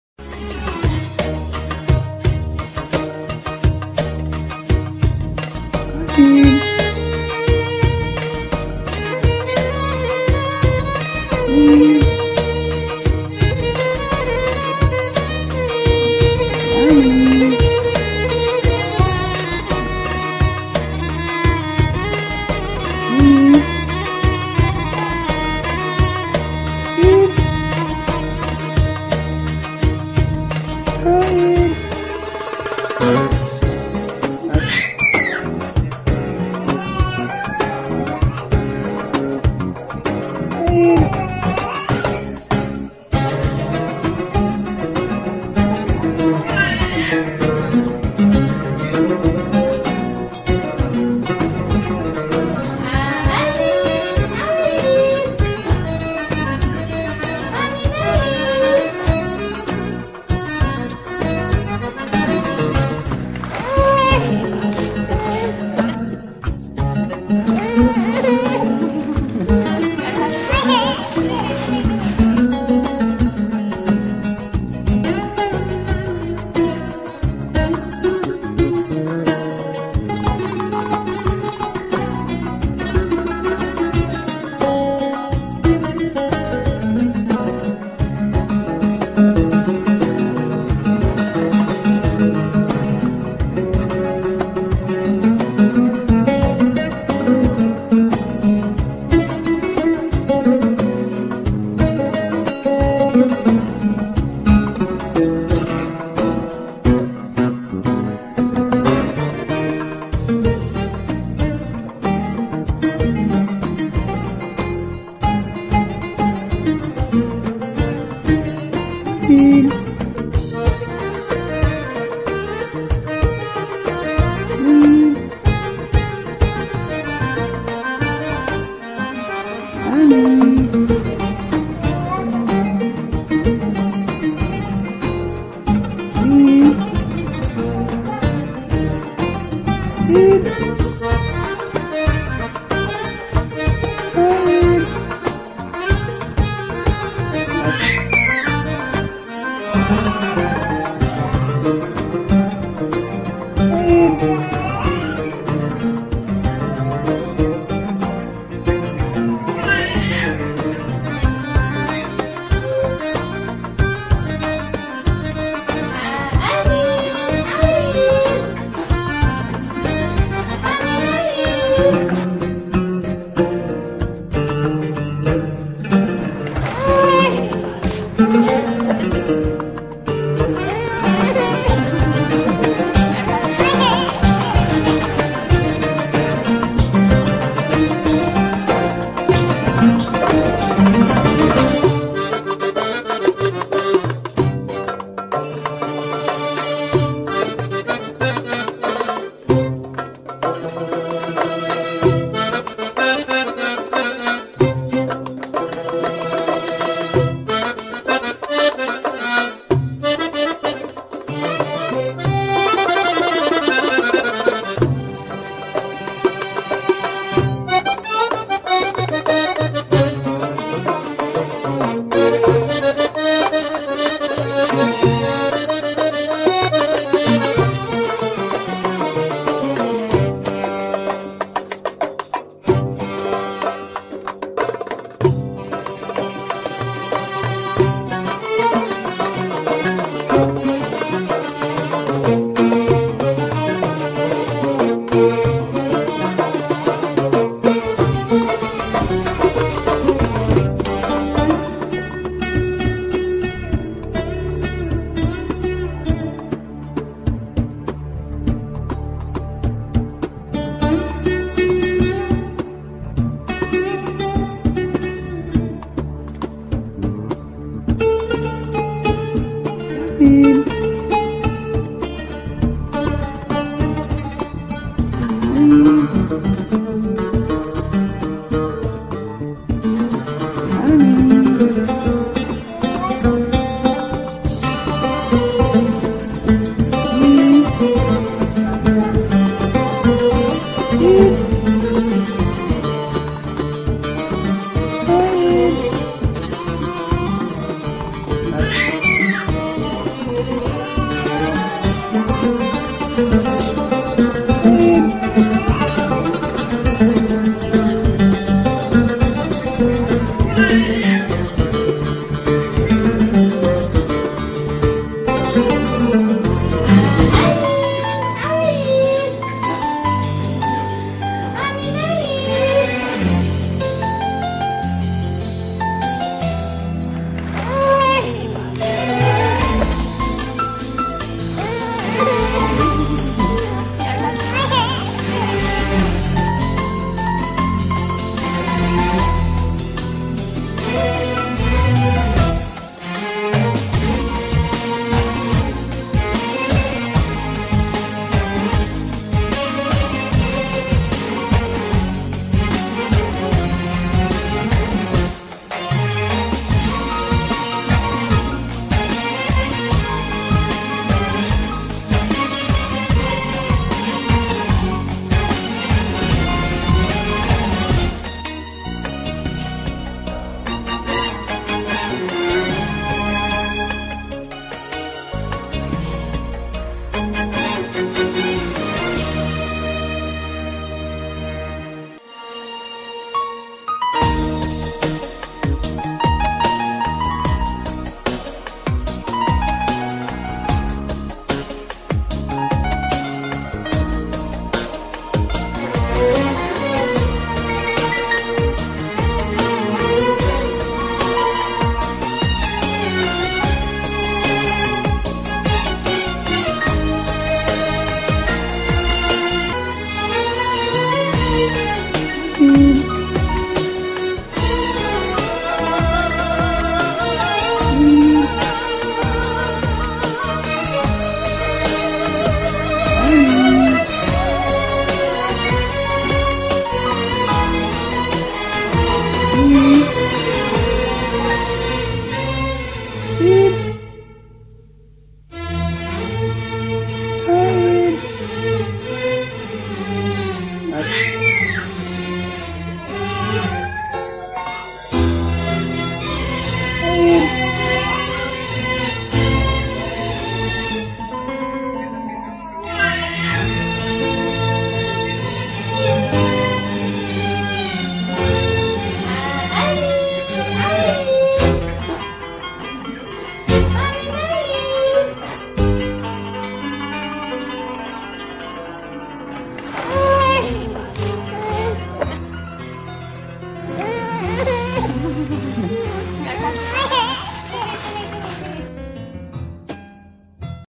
Slide photo Nurul Janin dan Nurul Hayyin putri kembar Fardhie, cuplikan ini di buat untuk menjadi kenangan bagi Anin dan Ayin saat mereka dewasa ©